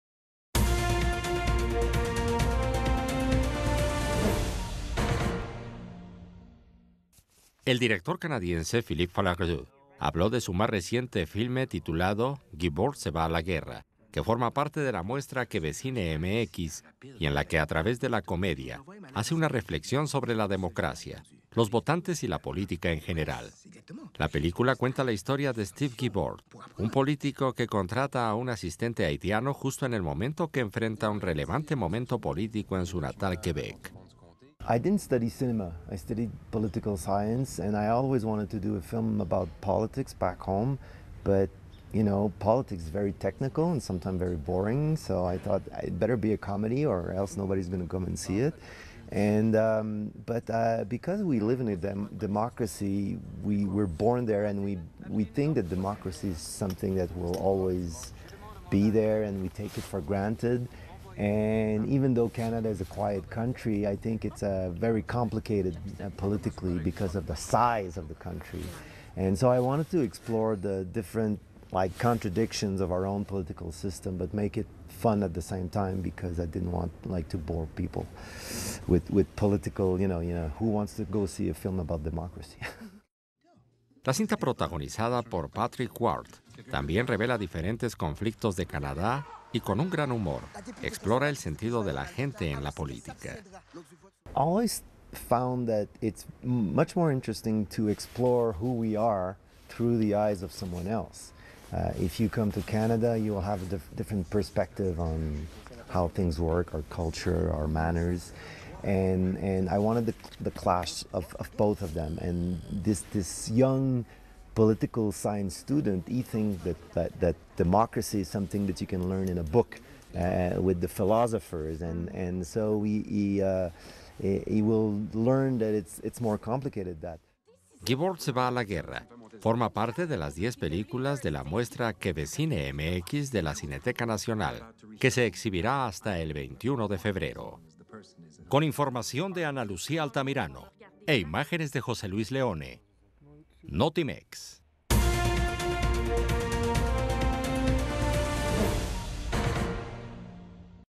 Guibord se va a la guerra reflexión sobre la democracia y la política a través de la comedia
El director canadiense Philippe Falardeau, habló de su más reciente filme titulado “Guibord se va a la guerra”, que forma parte de la muestra Quebecine Mx y en la que, a través de la comedia, hace una reflexión sobre la democracia, los votantes y la política en general.
El filme cuenta la historia de “Steve Guibord”, un político que contrata a un asistente haitiano justo en el momento que enfrenta un momento relevante en su natal Quebec, donde emprende un viaje para tomar decisiones que afectan a diversos sectores.